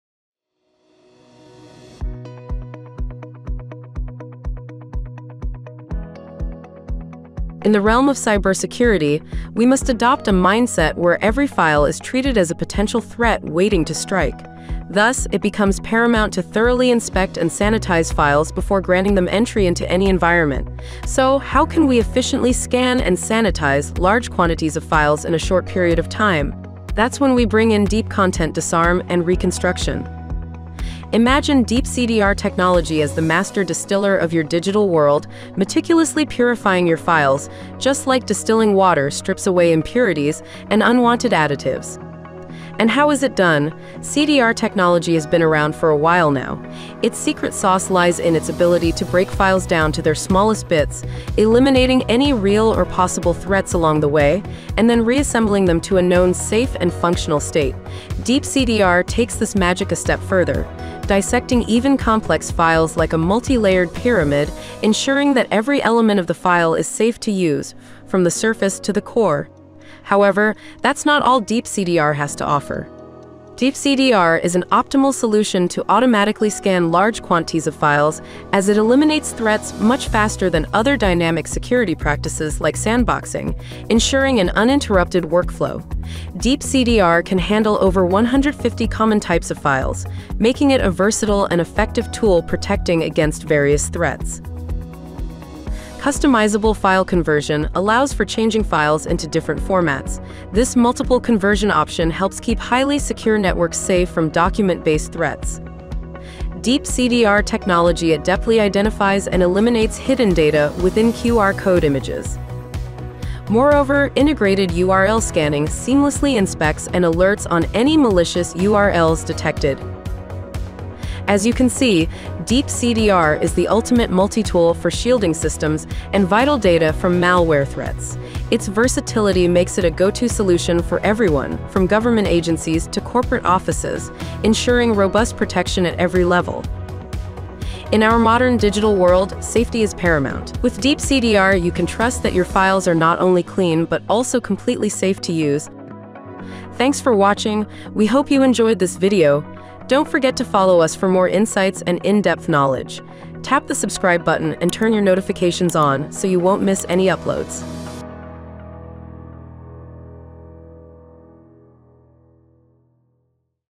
How Deep CDR Technology Works and Safeguard Your Organization
Information is particularly valuable nowadays, when even a piece can be worth millions of dollars.  Therefore, protecting sensitive data and systems is vital.

As cyber arms races rage on, it is vital to be armed with the latest technology. One of these defensive weapons is Deep CDR, developed to automatically detect and eliminate embedded threats in files by breaking them down, removing malicious elements, and rebuilding them to their latest known safe state.

Chapters
00:00 Introduction
00:58 Explaining Deep CDR
01:14 Benefits of Deep CDR
02:01 Real-World Application
💻 Become a part of the OPSWAT Academy community! Connect, engage, and stay updated: